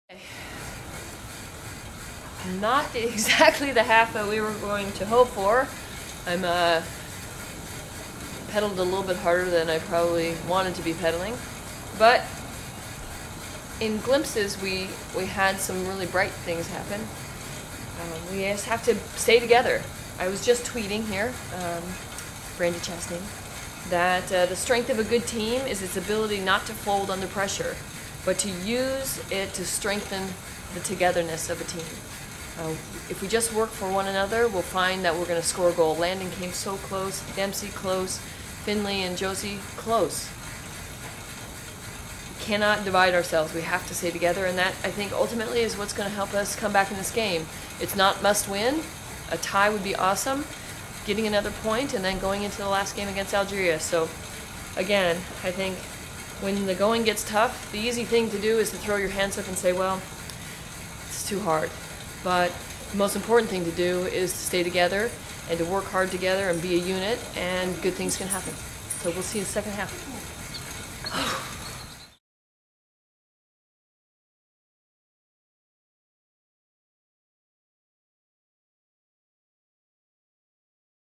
Brandi Watching World Cup USA-halftime report.mp4
Brandi pedaling her way through the game shares her thoughts. "The strength of  good team is it's ability not to fold under pressure."